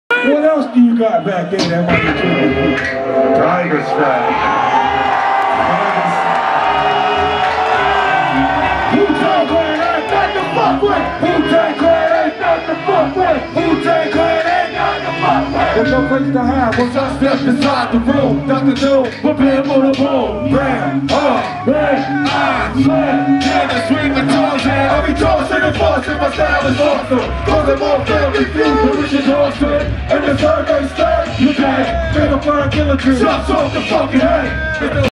Wu Years Eve 12/31/13 Method Man, Raekwon, Ghostface Killah
| Dec31.2013.Denver.CO Wu-Tang Clan members Raekwon, Method Man, & Ghostface Killah ring in the New Year at WU-YEARS EVE at Cervantes Masterpiece Ballroom with YOLO RUM !

Presented by YOLO RUM, LivWell, and Myxed Up Creations